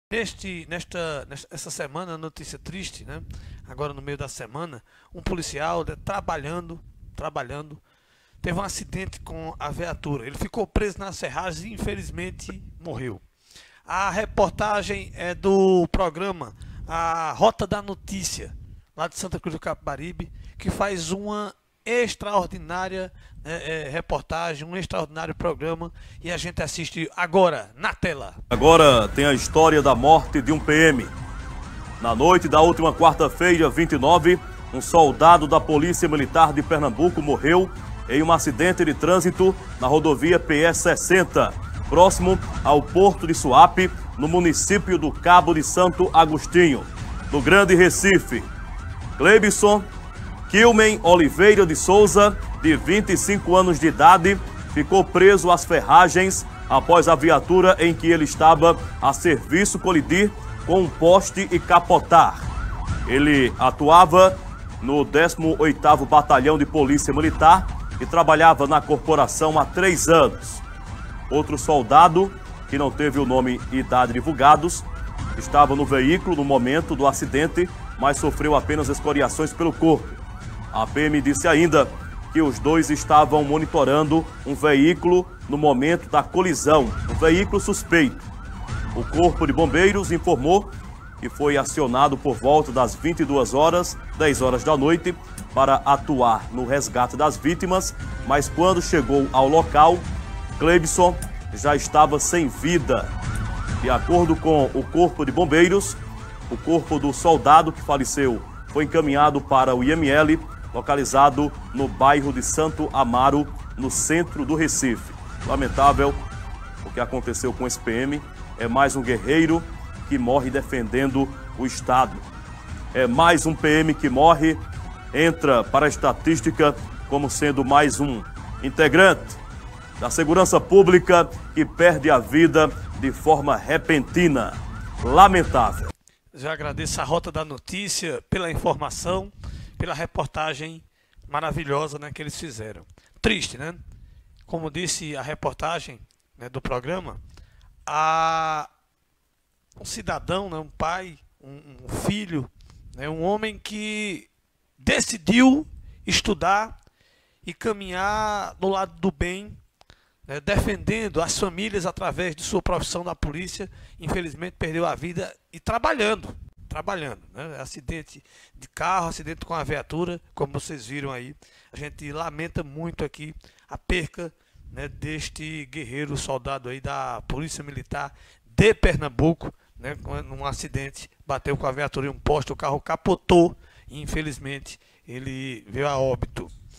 Policial militar morre em acidente na PE-60, no Cabo de Santo Agostinho
Um policial militar de 26 anos, identificado como Kilmen Sá, morreu em um acidente na PE-60, no bairro de Massangana, no Cabo de Santo Agostinho, na quarta-feira (29).

De acordo com testemunhas, ele conduzia uma viatura da PM e estava se deslocando para dar apoio a uma ocorrência policial quando perdeu o controle do veículo, que capotou na pista e bateu violentamente contra um poste.

Outro policial que também estava na viatura ficou ferido e foi levado para o Hospital Dom Hélder Câmara, no Cabo. Peritos do Instituto de Criminalística (IC) estiveram no local para saber as verdadeiras causas do acidente